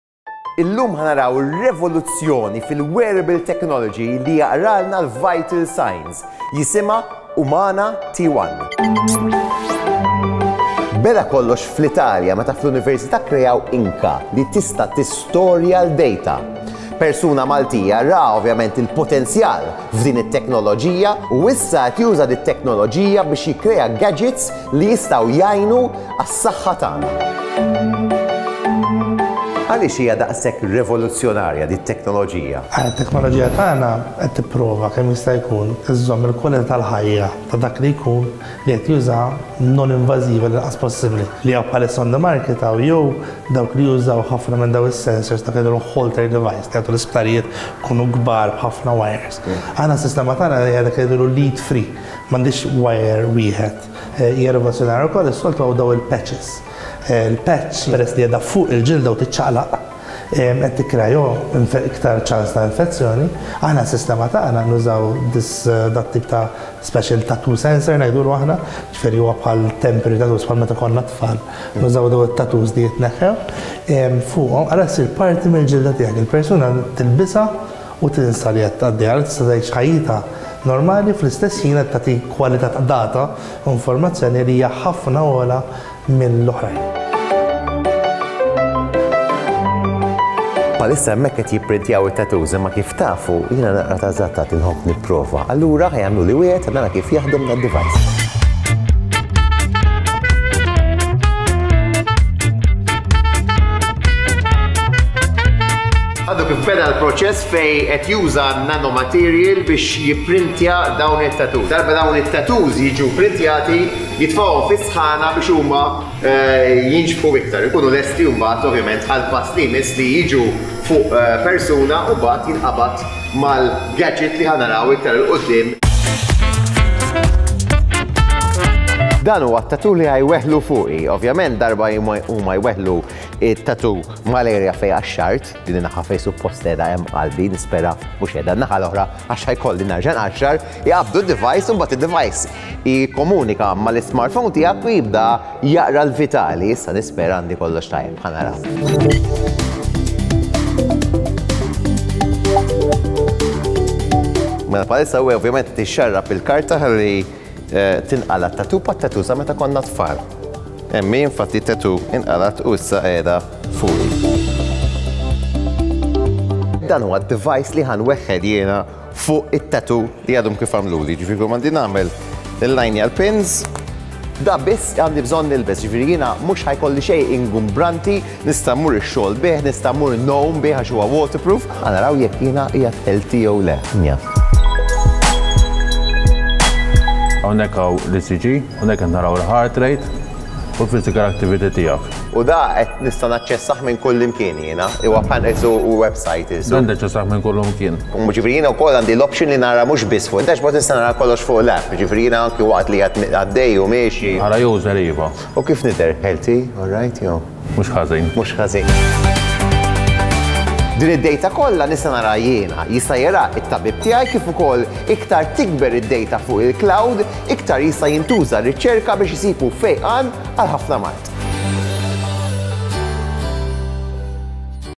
Umana: Tattoo Style Heart Rate Monitor
A new revolutionary introduction to wearable technology. The UMANA is a heart rate monitor that is wireless and communicates flawlessly with the sensor to monitor your heart rate. The sensor then connects to an app on your smartphone in order to give you analytics on your vitals. Newly created type of ink that is able to store data has made this type of tracking possible. The monitor is so easy and does not create any discomfort whatsoever, you don't even feel it, like a tattoo.